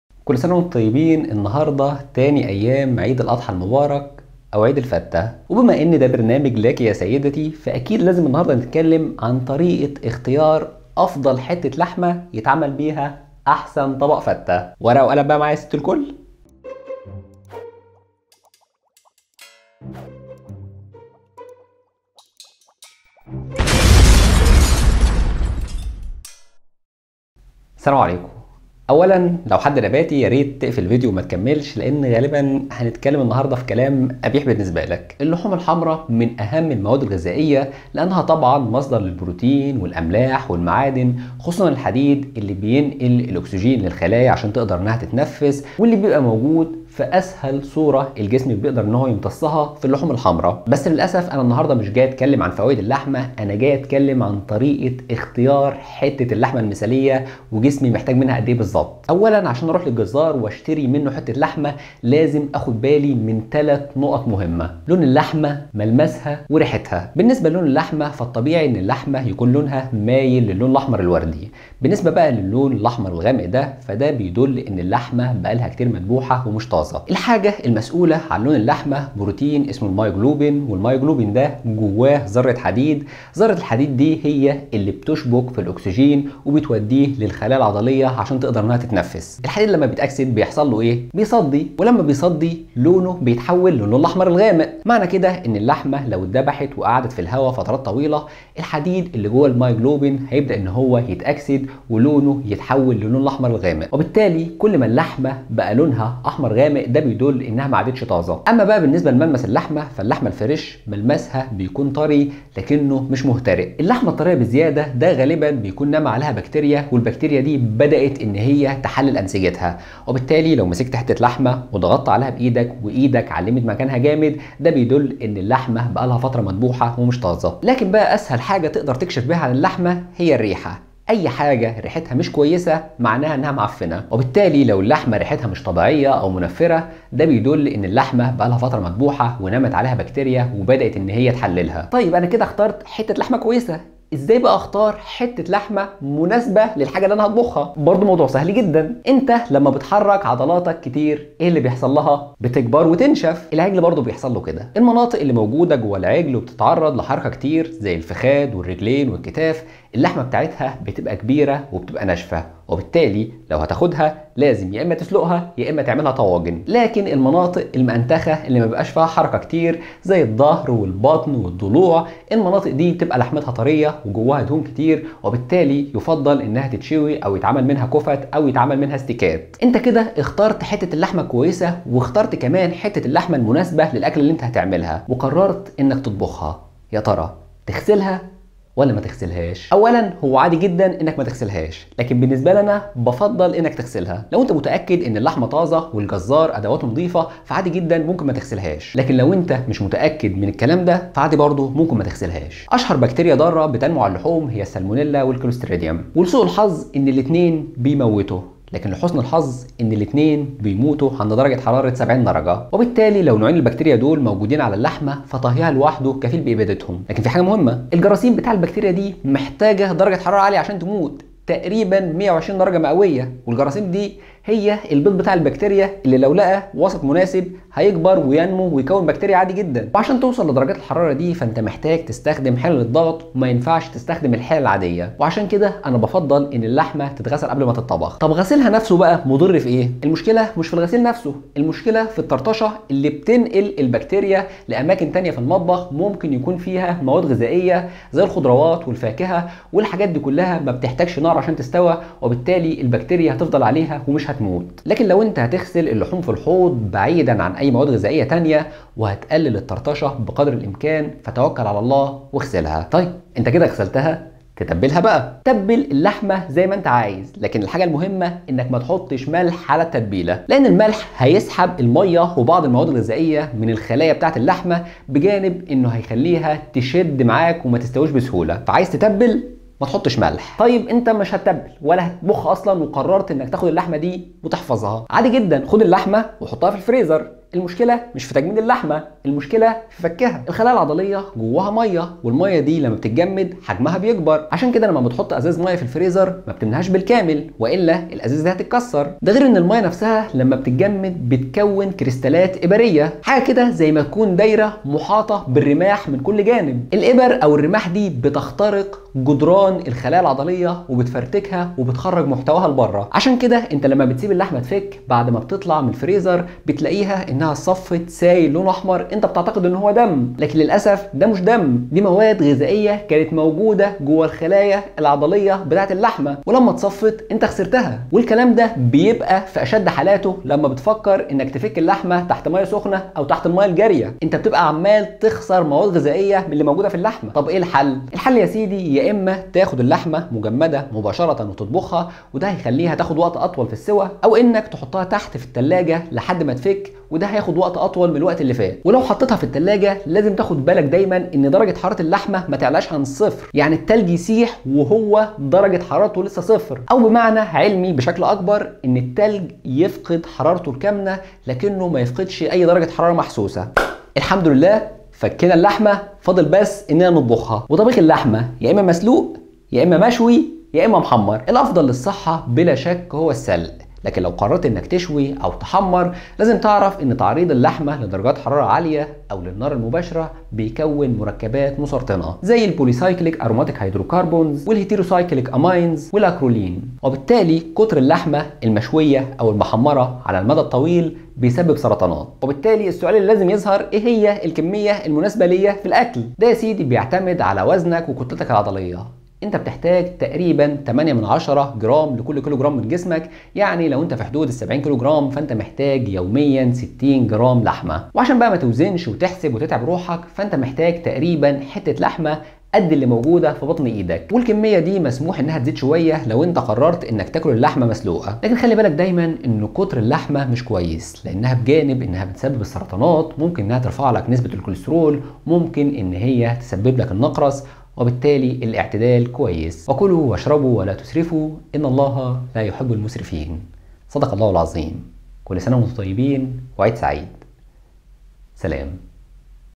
اللحوم الحمراء | طريقة اختيار اللحوم | دليل المستخدم
فيديو يوضح طريقه اختيار وتحضير وطهى اللحوم الحمراء  والكميات المطلوبه منها